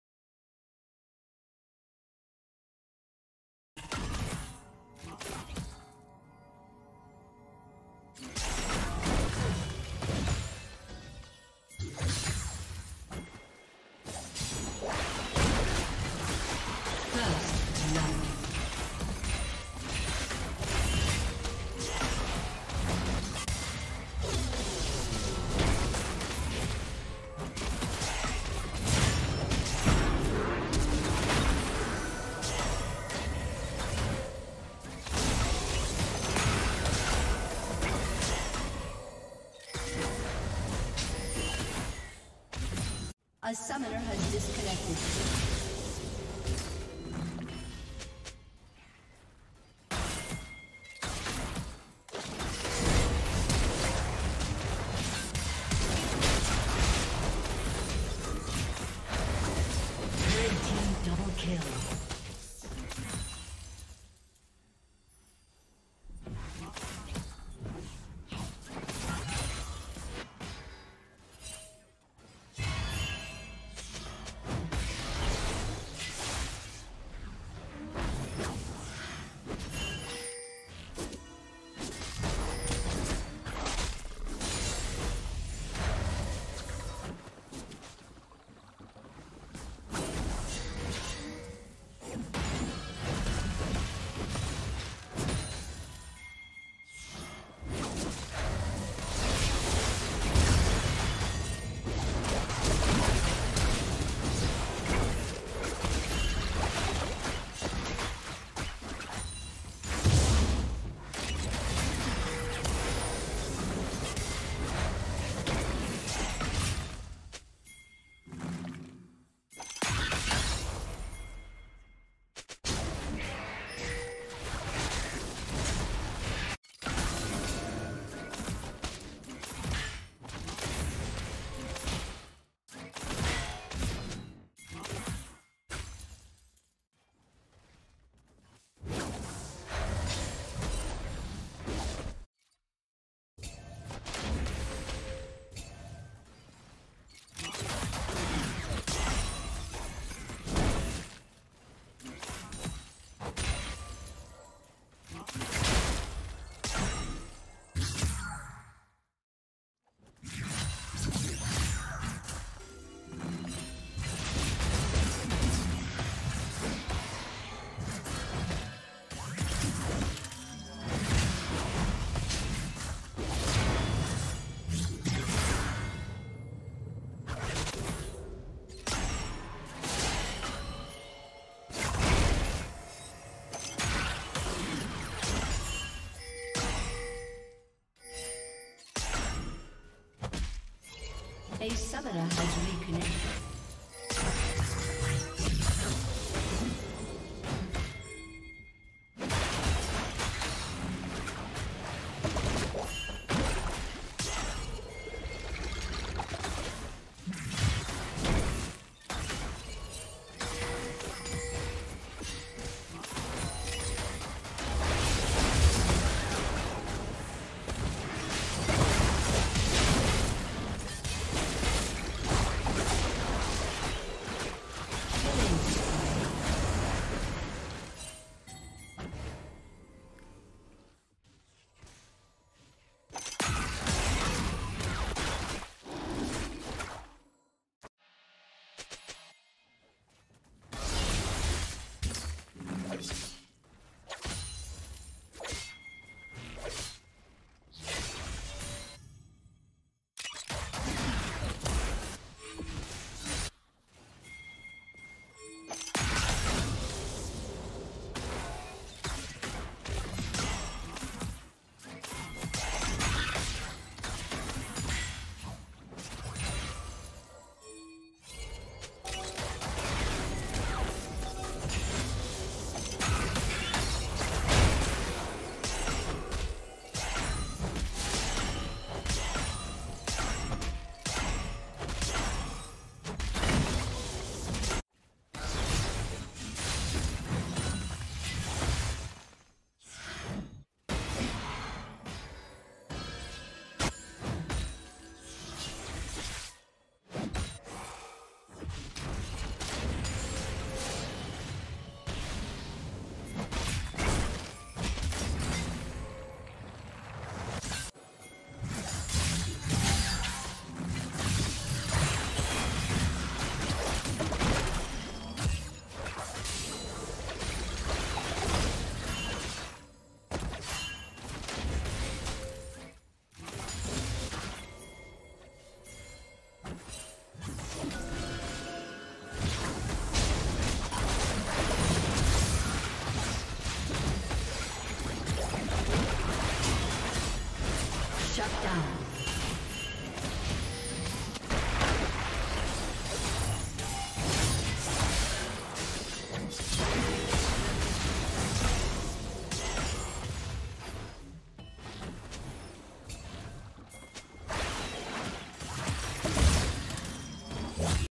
[2021-09-27 #67238] Pros: DKC Chasy, KT Doran, LSBA Kabbie, KT Dove, DKC Muru
Watch KT Doran, KT Dove, LSBC Kabbie, DKC Chasy, DKC Muru, Dalka in solo queue